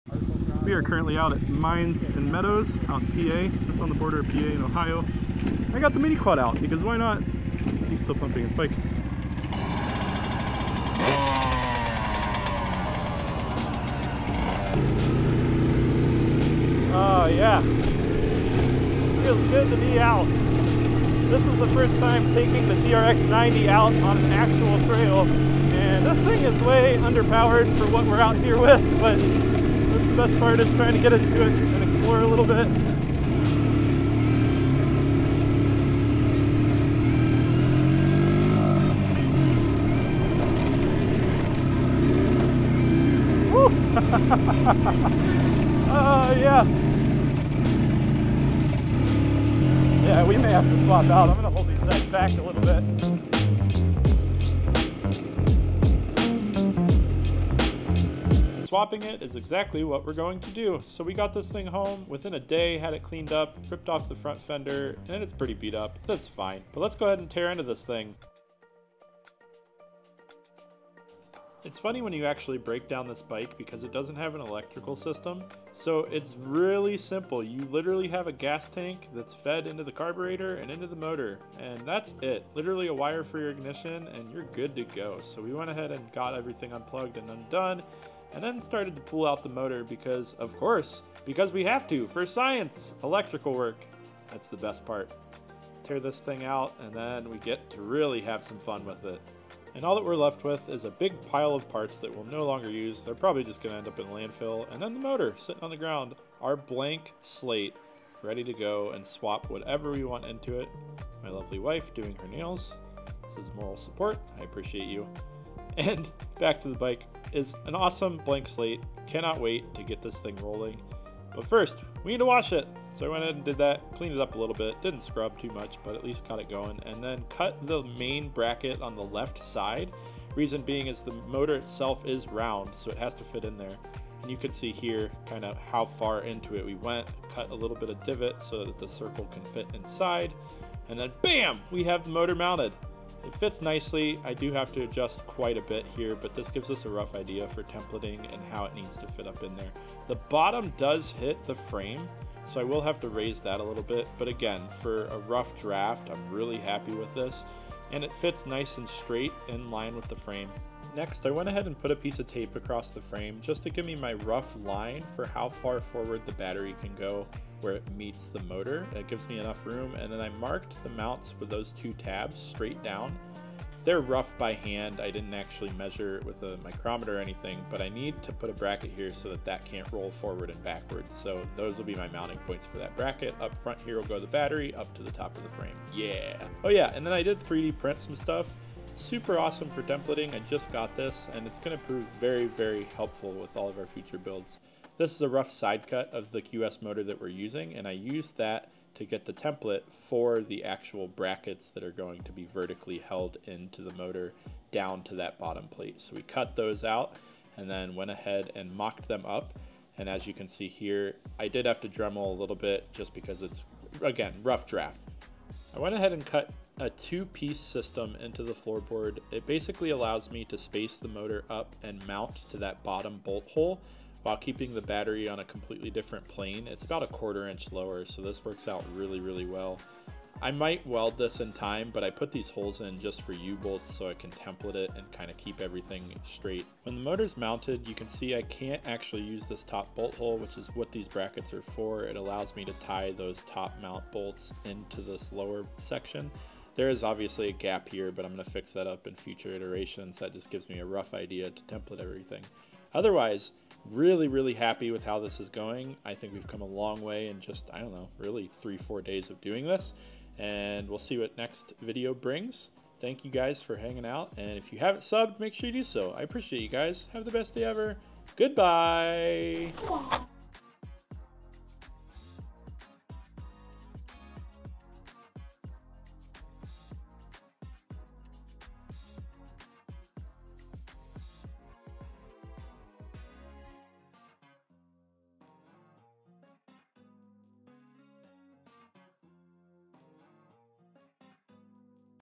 Building an Electric Mini Quad; Electric Honda TRX90
In this video, I’m kicking off a brand new series where I convert a Honda TRX90 into a fully electric quad! We’ll be going through the entire electric swap process — from teardown and planning, to motor installation, wiring, and testing. If you've ever thought about electrifying a mini quad, this is the series for you.

👉 Got questions or suggestions for the build? Drop them in the comments below!